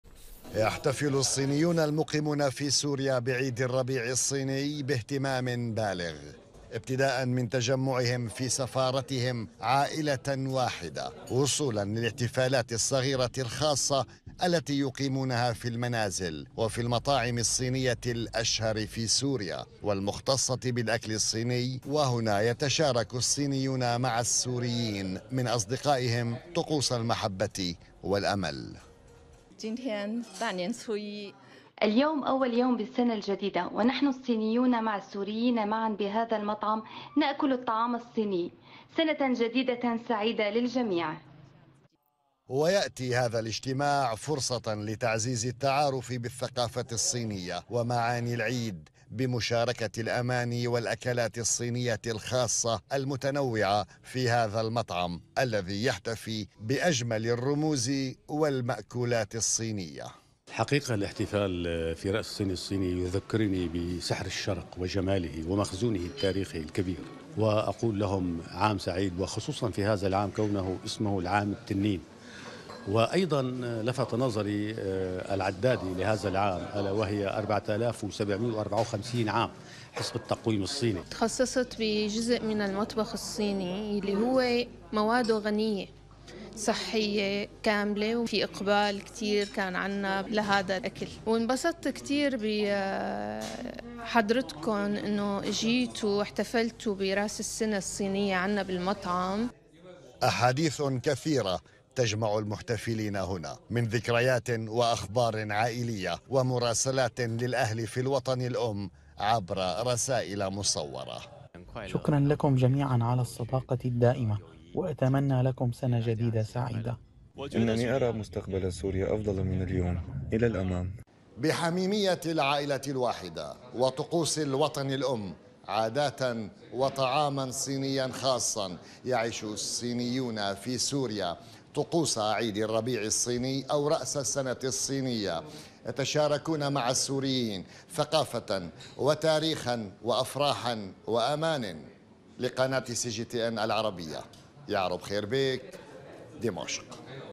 احتفالات الصينيين مع السوريين في سوريا بعيد الربيع
يتزايد الاهتمام الدولي باحتفالات عيد الربيع الصيني. وفي سوريا أقبل السوريون على المطاعم الصينية لمشاركة الجالية الصينية أجواء العيد.